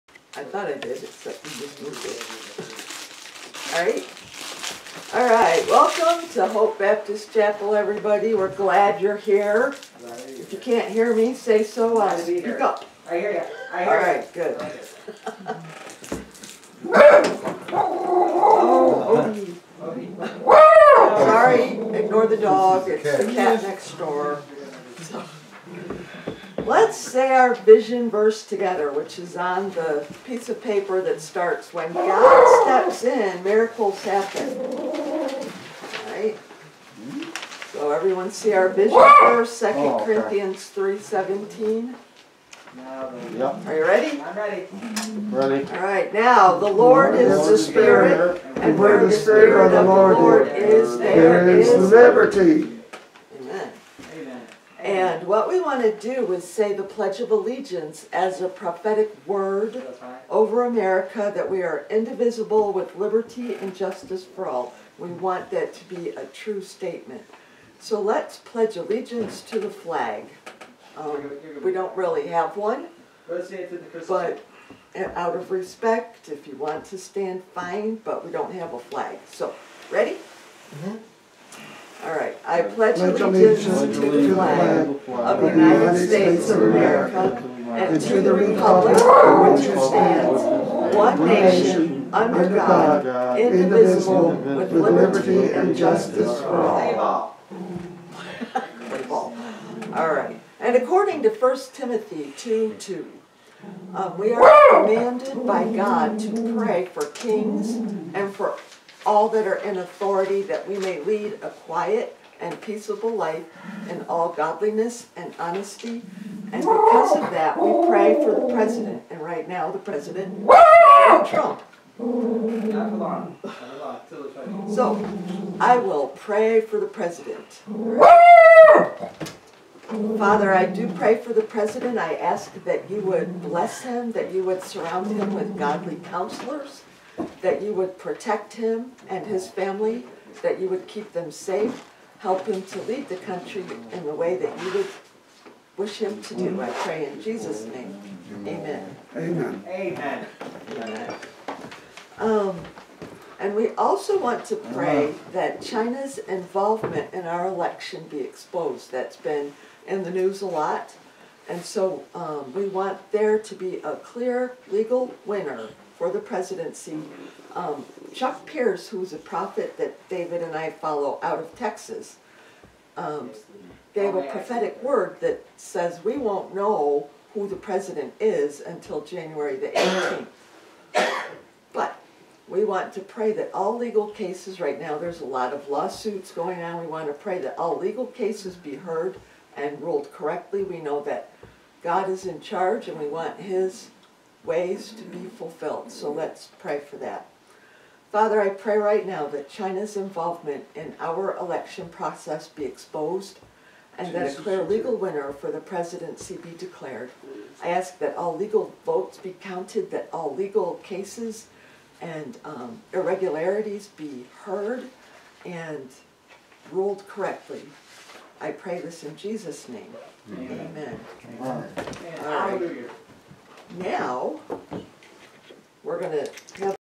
Opening Prayers
Vision Verse 2 Cor. 3:17, Pledge of Allegiance, Pray for the President according to 1 Timothy 2:2, and Prayer that China’s involvement in our election to be exposed, and a clear legal winner for the presidency to be declared.